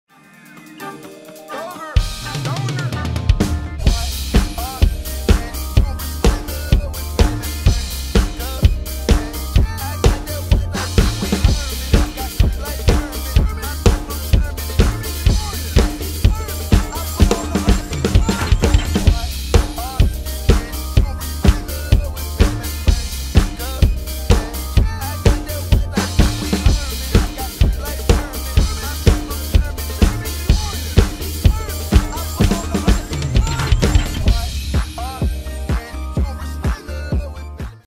the vivi trend (drum reinterpretation)
i'm using :
• electric drum / NUX DM5s
* soundcard / midiplus studio S
• daw / studio one
• vst / ezdrummer 2
• iem / that 75k rupiah iem from minisoo
• kick pedal / yamaha FP-7210A
• drumstick / Maple 5A